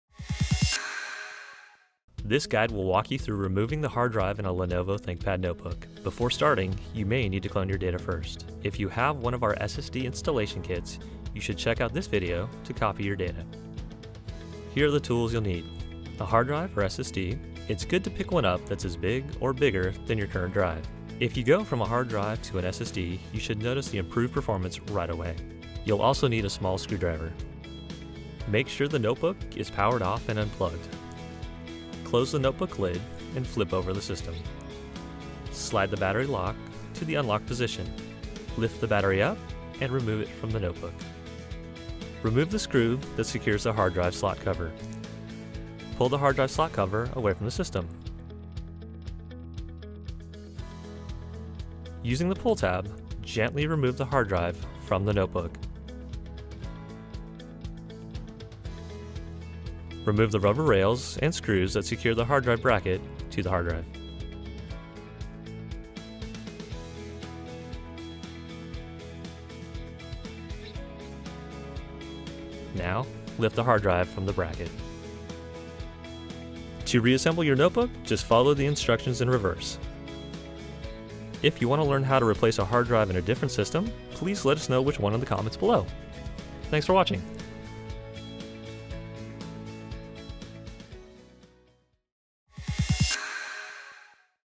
How to Install an SSD in Lenovo Thinkpad (T410/T420/T430) - Kingston Technology
This guide will walk you through removing the hard drive in a Lenovo Thinkpad T410, T420, or 430 notebook.  If you have one of our SSD installation kits you can clone your HD data first so you should wach this video also before your start

Tools:
• A hard drive or SSD. It’s good to pick one up that’s as big, or bigger, than your current drive.
• Small screwdriver

Step 1 – Make sure the notebook is powered off and unplugged. Make sure the notebook is powered off and unplugged. Close the notebook lid and flip the system over.  Remove the battery.

Step 2 –Remove the screw that secures the hard-drive slot cover. Pull hard-drive slot cover away from the system.  Using the pull tab, gently remove the hard-drive from the notebook.  Remove the rubber rails and screws that secure the hard drive bracket to the hard drive.  Lift the hard-drive from the bracket to remove.

Step 3 - Reassemble your notebook by follow the instructions in reverse.  Attach the screws and rubber rails to the SSD and insert it into the bracket.  Connect the cable and slide the drive into the slot.  Screw the cover back on.  Reattach the battery.